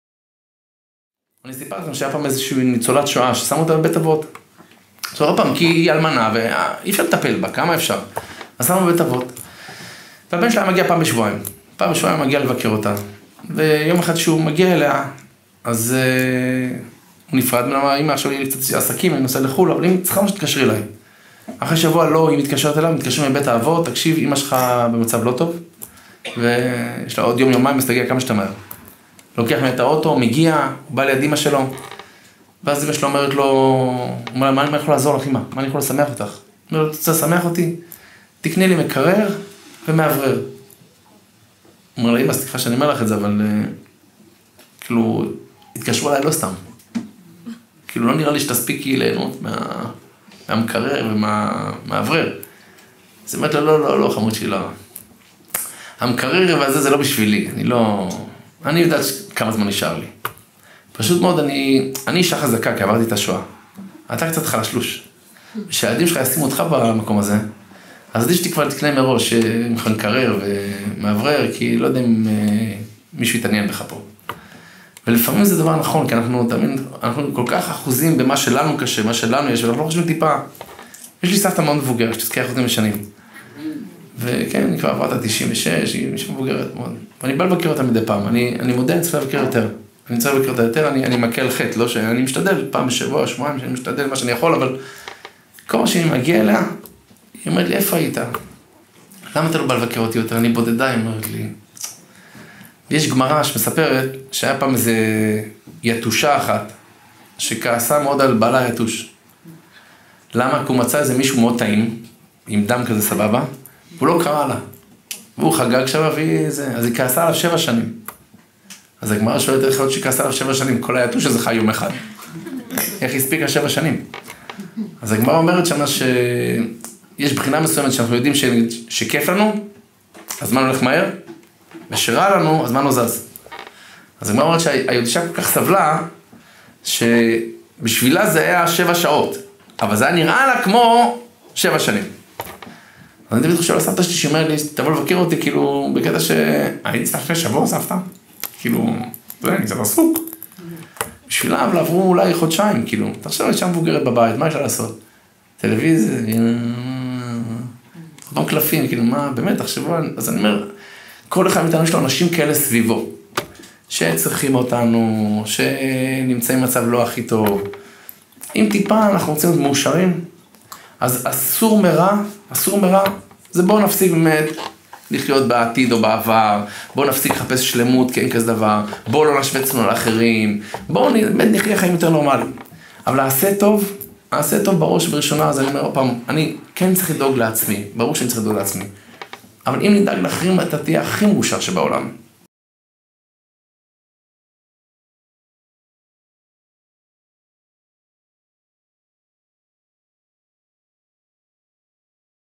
הרב יצחק פנגר – רוצה להיות מאושר? תתחיל לדאוג לאחרים!
יש לך סבתא בבית אבות? אבא שזקוק לך? עשה טוב. זה יעשה אותך מאושר. צפו